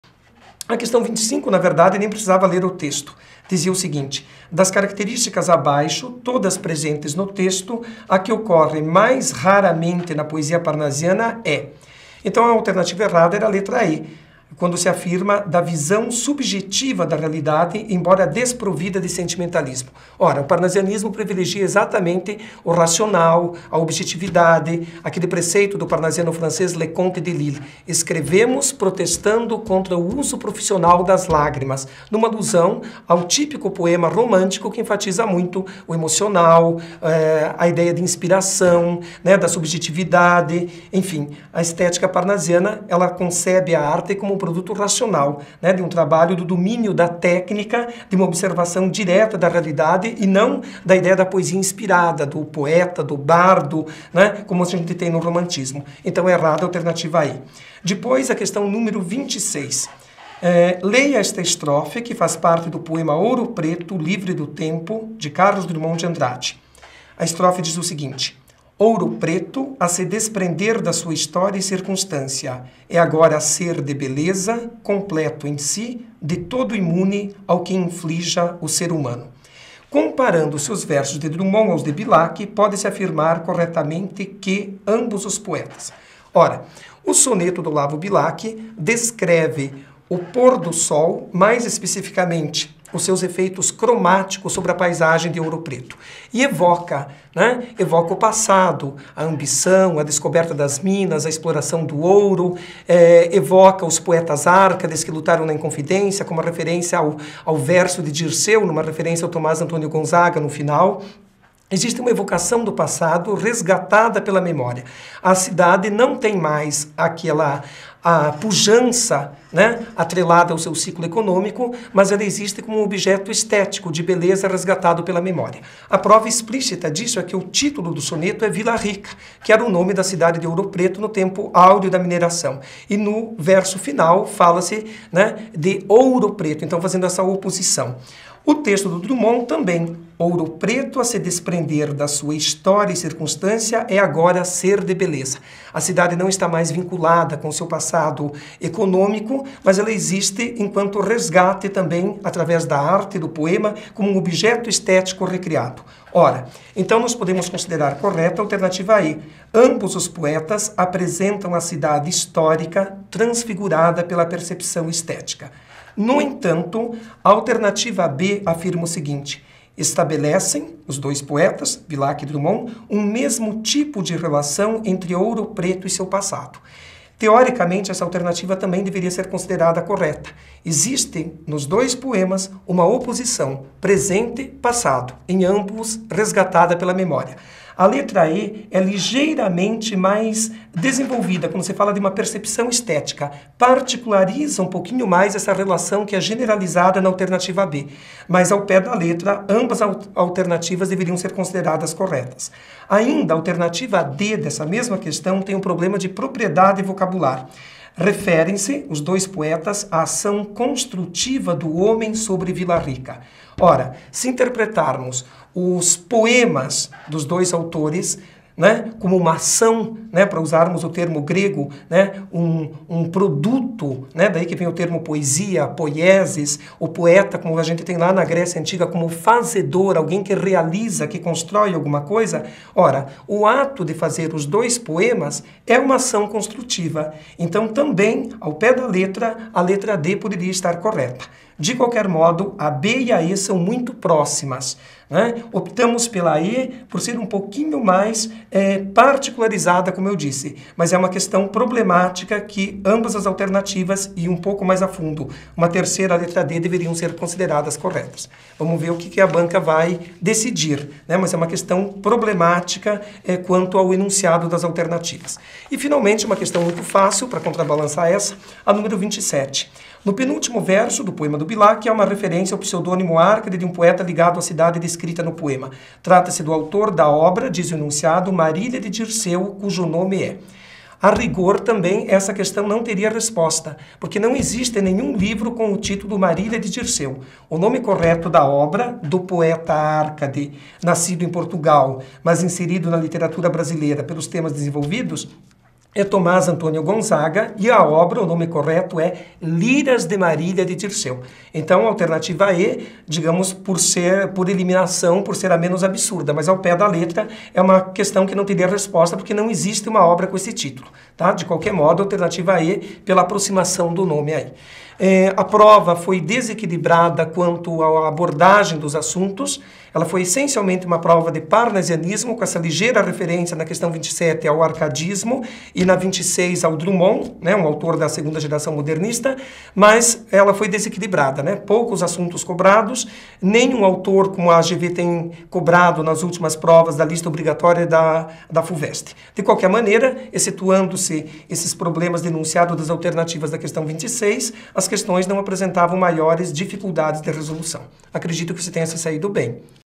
FGV Adm 2010 1º Semestre - Literatura (Parte 2)
Comentário do Professor do CPV para o vestibular FGV Administração 2010 1º Semestre.